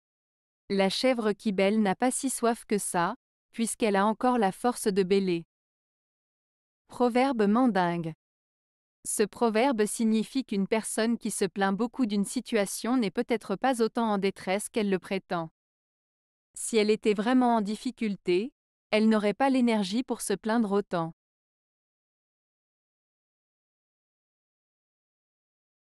La chèvre qui bêle n'a pas si soif que ça, puisqu'elle a encore la force de bêler.
Ce proverbe signifie qu'une personne qui se plaint beaucoup d'une situation n'est peut-être pas autant en détresse qu'elle le prétend. Si elle était vraiment en difficulté, elle n'aurait pas l'énergie pour se plaindre autant.